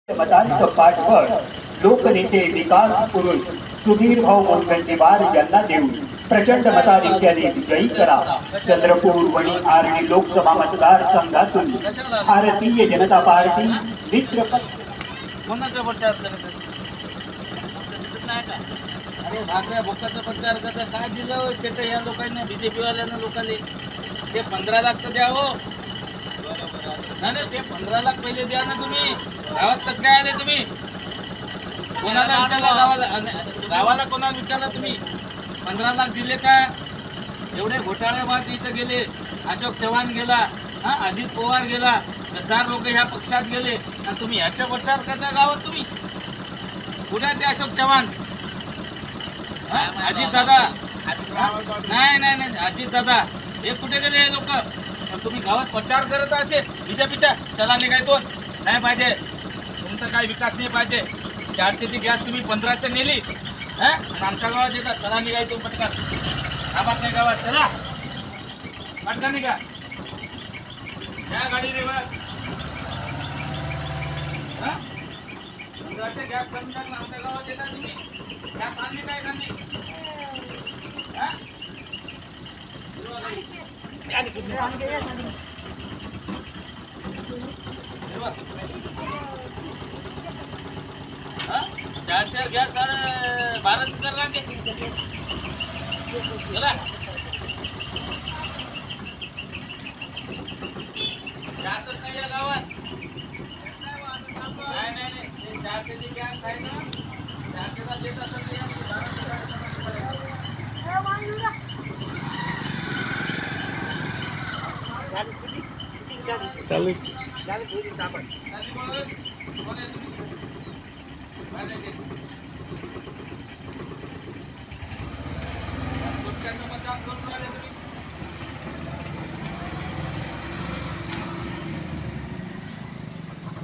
Chandrapur Lok Sabha | Sudhir Mungantiwar | मुनगंटीवारांच्या प्रचाराची गाडी तरुणाने हकलून लावली
15 लाखांचं काय झालं? चारशे रुपयांचा गॅस पंधराशे रुपयांवर गेला म्हणत सुधीर मुनगंटीवार यांच्या प्रचारासाठी आलेली गाडी हकलून लावतानाचा एक व्हिडिओ सोशल मिडियावर तुफान व्हायरल होत आहे. आर्णी तालुक्यातील खेडी पुरुषोत्तम नगरमधील हा व्हिडिओ असल्याचं बोललं जात असून एक तरूण वाहन चालकाला गावातून निघून जाण्यासाठी सांगत असल्याचं या व्हिडिओत दिसतंय.